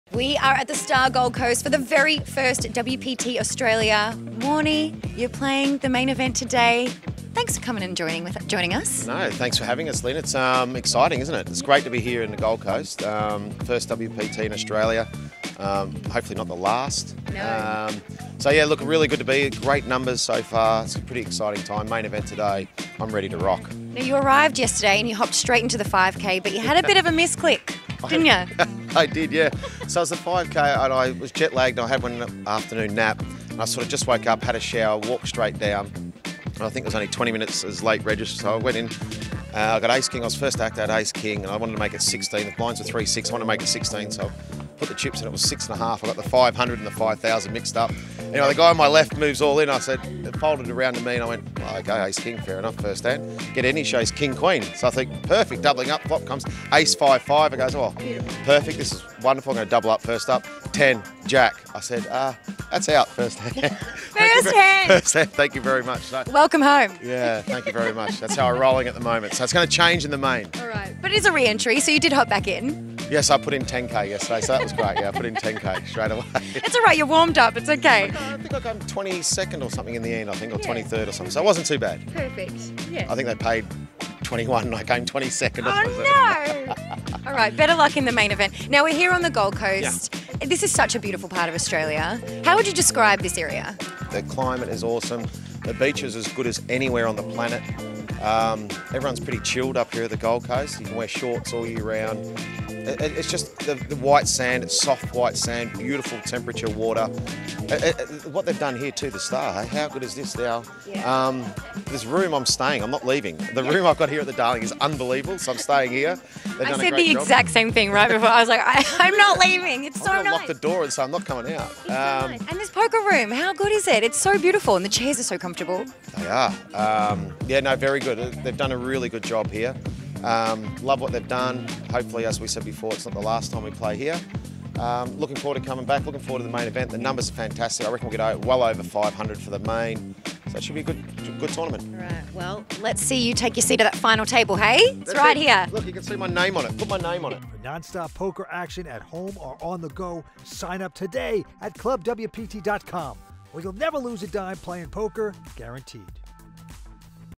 Lynn Gilmartin Chats with Shane Warne at WPT Australia
Shane Warne plays in the first-ever WPT event in Australia. Lynn Gilmartin catches up with Shane at The Star Gold Coast.

Give us love on Instagram